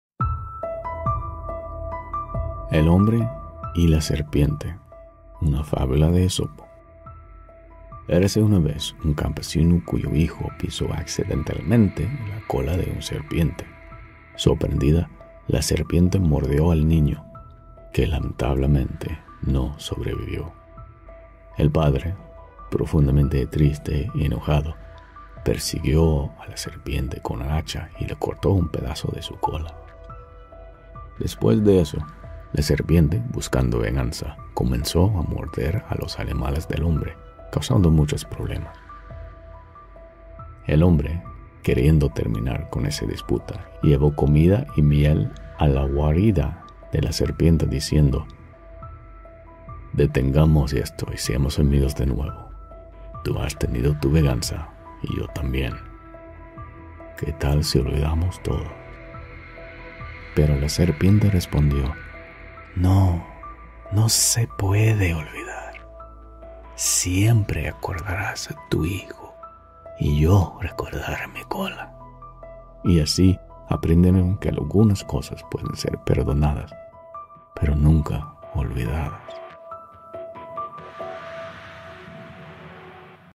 020_El Hombre y la Serpiente - Una Emocionante Fábula de Esopo
"Descubre el cuento milenaria que aún fascina: 'El Hombre y la Serpiente', una de las fábulas más conmovedoras de Esopo. En este video, nos sumergimos en la narrativa cautivadora de un campesino y una serpiente, desvelando lecciones sobre venganza, pérdida y el poder del perdón. Observa cómo un simple acto desencadena una cadena de eventos que cambiará para siempre la vida de estos personajes.

Este cuento no solo entretiene, sino que también ofrece reflexiones profundas sobre la naturaleza humana y las consecuencias de nuestras acciones. Ideal para todas las edades, el video presenta una animación cautivadora y una narración envolvente que captura la esencia de esta historia atemporal.

No pierdas la oportunidad de emocionarte y aprender con 'El Hombre y la Serpiente'.